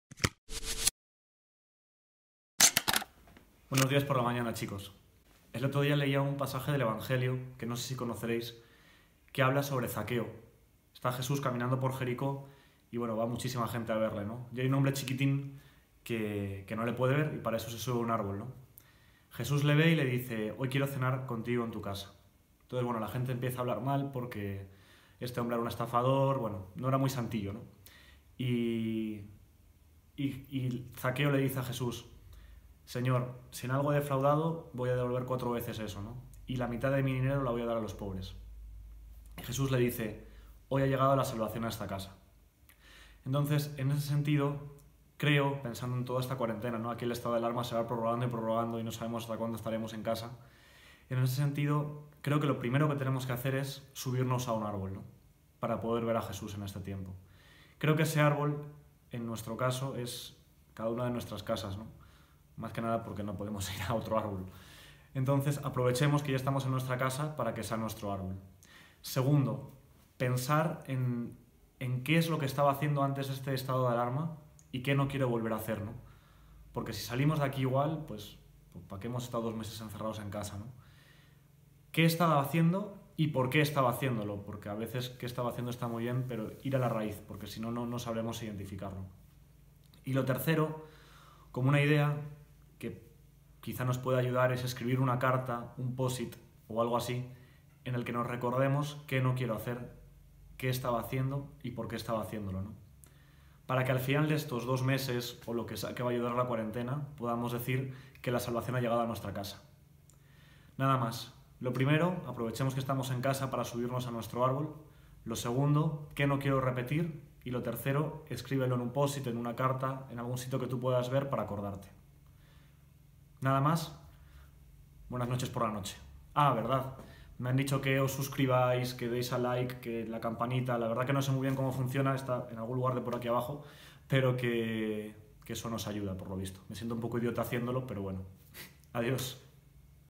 SEMPER ven y verás💥▶️ HOY ha llegado la SALVACIÓN a esta casa 🏡
¡Ojalá cuando acabe la cuarentena podamos decir como Zaqueo "hoy ha sido la salvación de esta casa"!

- Súbete a tu árbol para ver pasar a Jesús
- Identifica qué cosas previas a la pandemia no quieres repetir
- Escribe algo que te sirva de recordatorio

👋Ven y Verás💥🏡